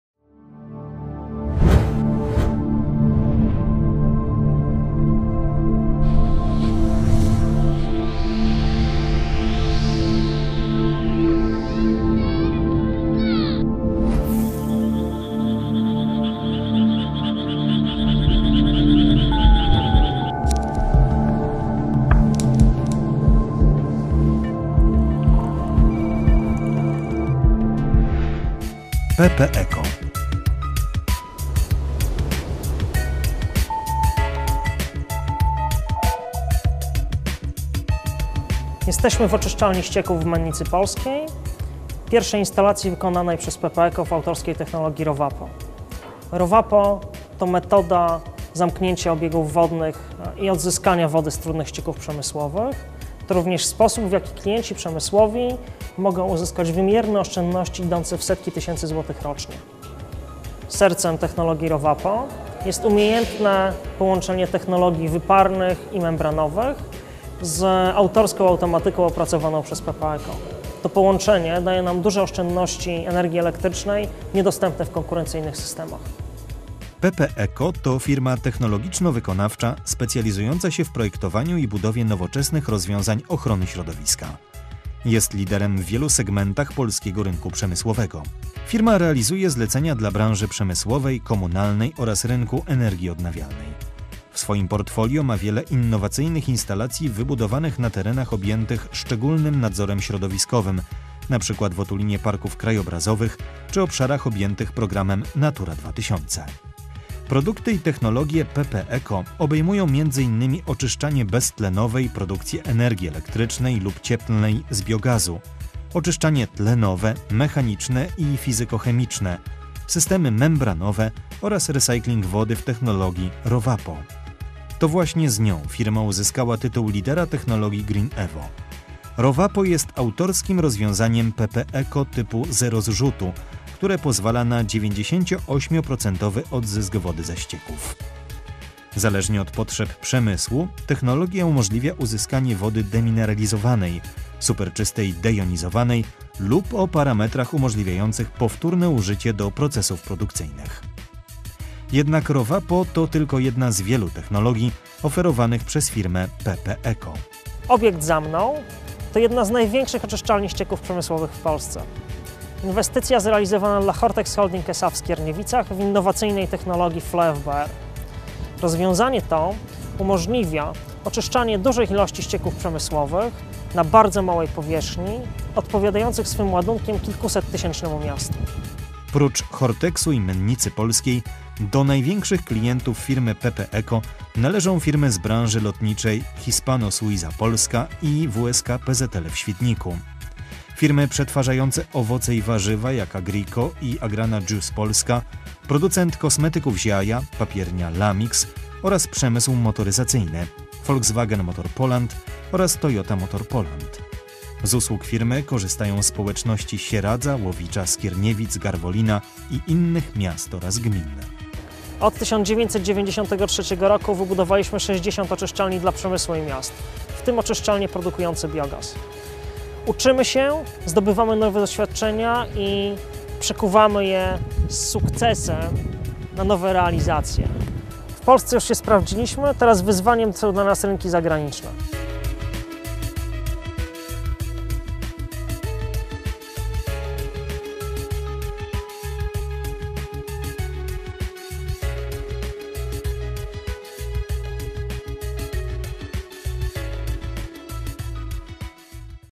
PP-EKO - oczyszczanie ścieków przemysłowych/industrial sewage treatment plants
PL/ENG

Laureat I edycji rządowego projektu GreenEvo, prowadzonego przez Ministerstwo Środowiska.

Technologia: Zero zrzutu, zero odpadów, zero problemów z czystą wodą odzyskaną ze ścieków przemysłowych. Tak jednym zdaniem można opisać technologię ROVAPO™, która pozwala na produkcję bardzo czystej wody technologicznej. ROVAPO™ jest technologią zamkniętego obiegu wodnego, która minimalizuje ilość zużytej energii oraz powstających odpadów. To technologia tak czysta jak woda, którą dzięki niej uzyskujemy. ROVAPO™ to również wysoka wartość wskaźnika zwracania wody do powtórnego użycia (do ponad 95%).

Winner of the 1st edition of the GreenEvo project, run by Ministry of the Environment, Poland.

Technology: No emissions, no waste, no problems with clean water recovered from industrial waste products. In this one sentence you can describe the technology of ROVAPO™, which enables the production of very clean technological water. ROVAPO™ is a closed water circulation technology that minimizes energy consumption and resulting waste products. It is a technology as clean as the water it generates. ROVAPO™ also means a high level of water recycled for repeat use (up to 95%).